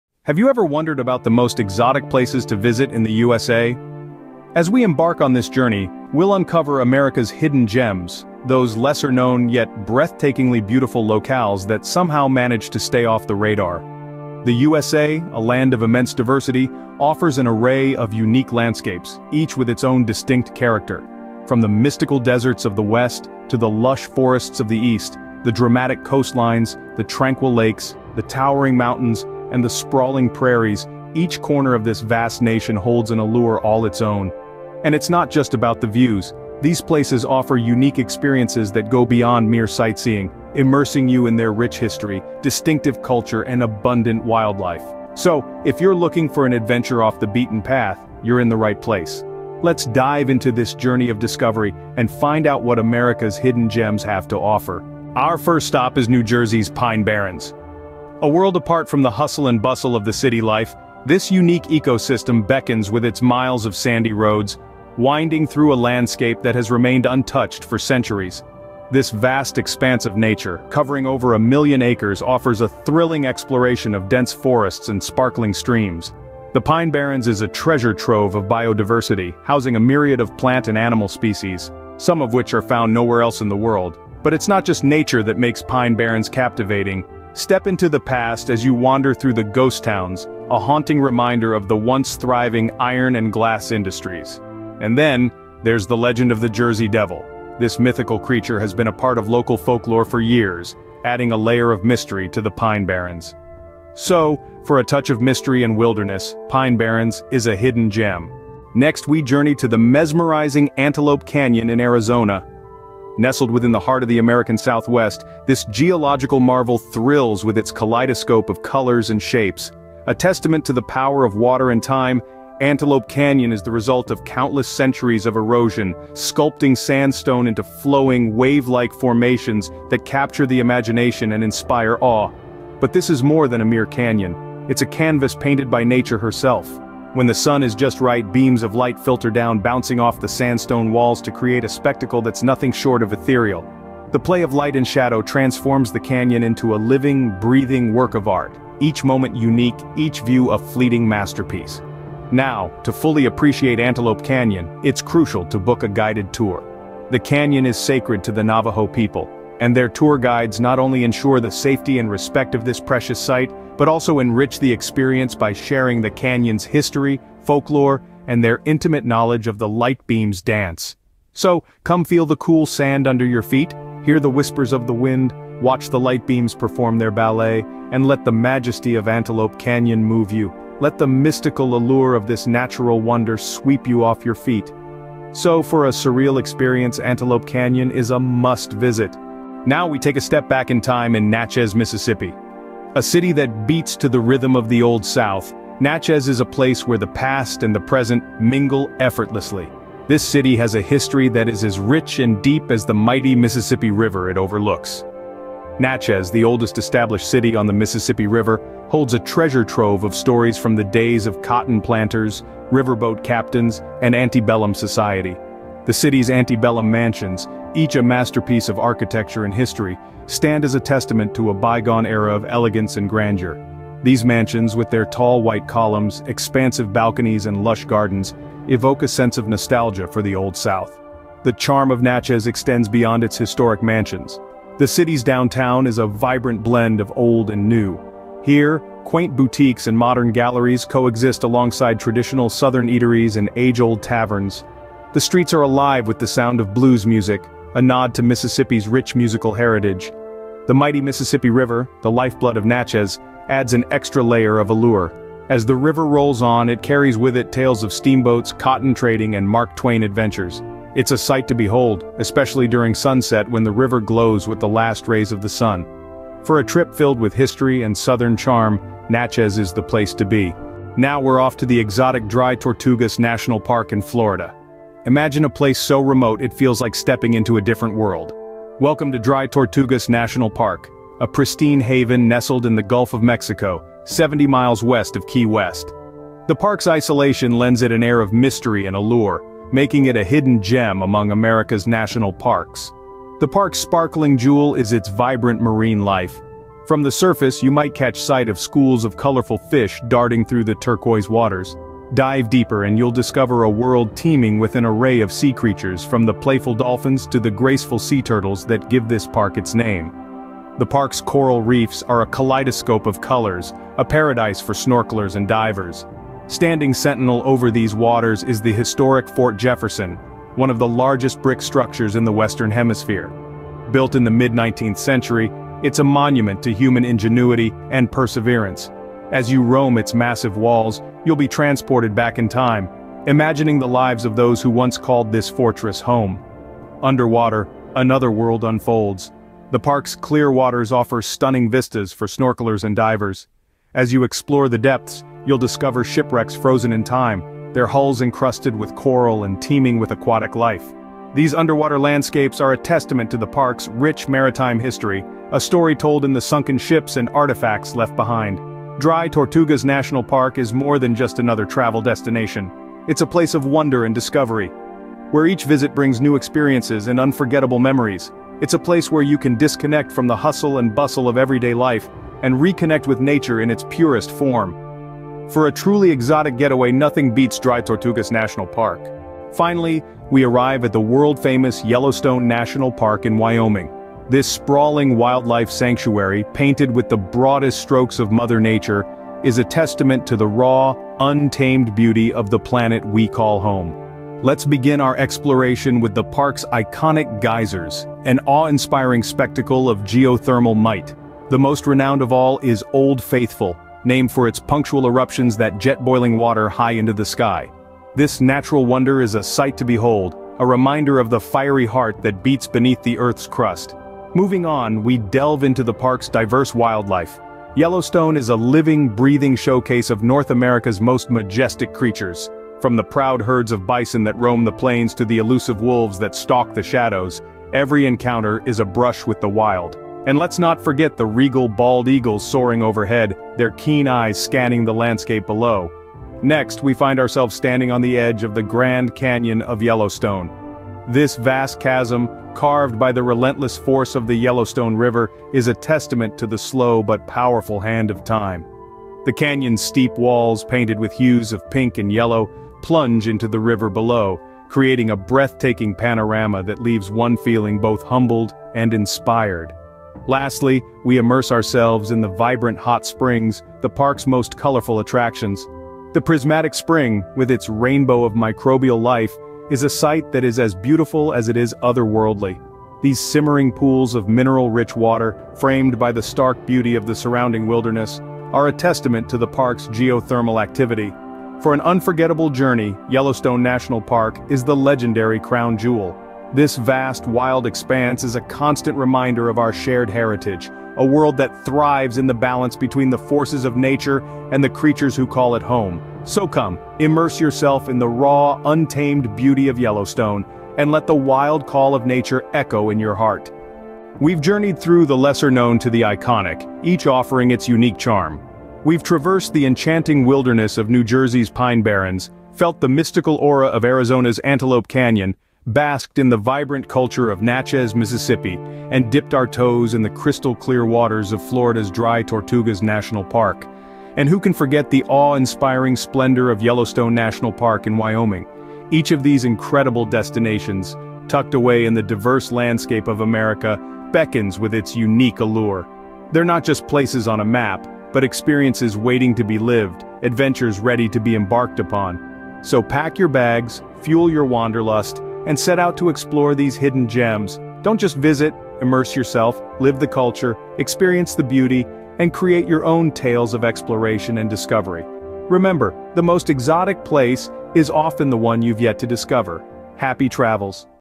Exotic Places To Visit In The USA | Travel Video
Embark on a journey through the breathtaking landscapes of the United States with our travel video showcasing exotic destinations you must visit! From the stunning beauty of the Grand Canyon to the lush greenery of the Hawaiian Islands, we uncover hidden gems and off-the-beaten-path locations that will leave you awe-inspired. Join us as we wander through vibrant cities, explore serene national parks, and immerse ourselves in the rich cultural tapestry of America. Whether you're seeking adventure or tranquility, this video will guide you to discover the diverse and exotic wonders awaiting you in the USA. Don't miss out on these unforgettable experiences!

00:00:00 Uncovering America's Hidden Gems
00:00:55 The Enchanting Pine Barrens, New Jersey
00:01:48 The Mystical Antelope Canyon, Arizona
00:03:09 The Vibrant Natchez, Mississippi
00:04:30 The Magical Dry Tortugas National Park, Florida
00:06:19 The Iconic Yellowstone National Park, Wyoming
00:08:23 Wrapping up the Exotic Journey